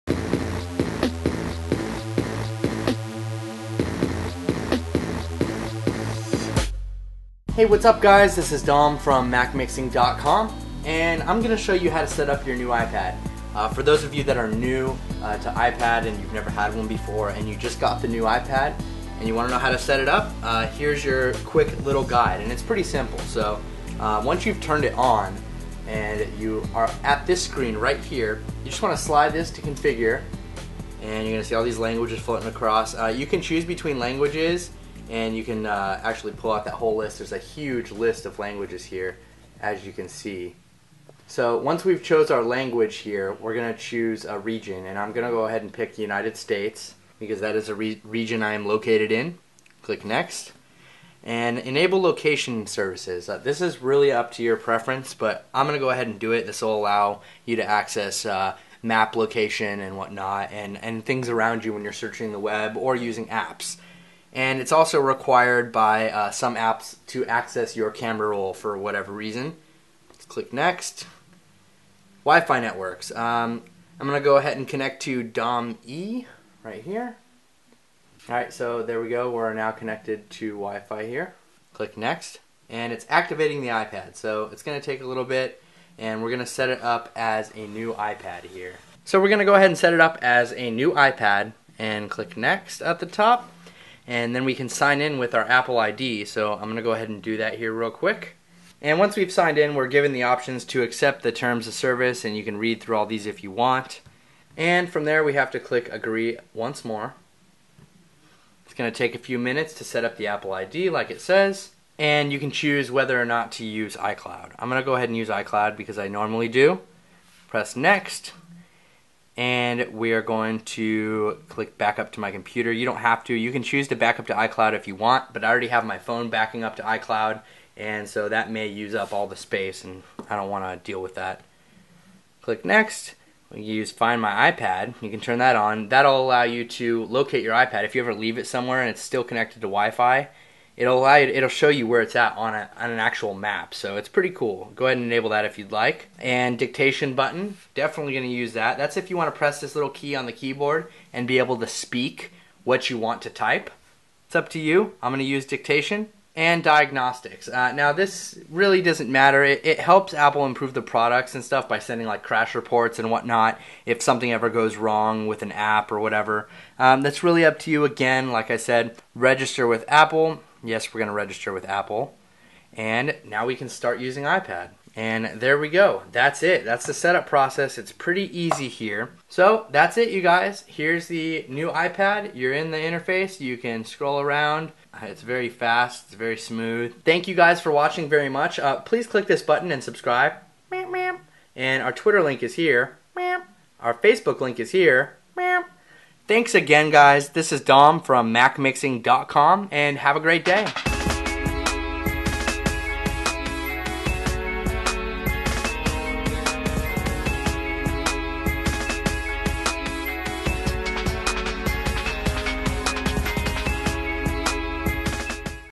[How-To] Setup a New iPad - 3rd Generation iPad
- Mixing it up with Apple
New to iPad? If you just bought a new iPad and need help getting it setup be sure to check out this video. The new iPad is a great addition to your family. With a few simple steps, you can have your iPad up and running. The entire process can be done without a computer right on your iPad.

The New iPad comes equip with Retina Display, 1GB of RAM, a faster A5X Processor, Quad-Core Graphics, 4G LTE, and more!

The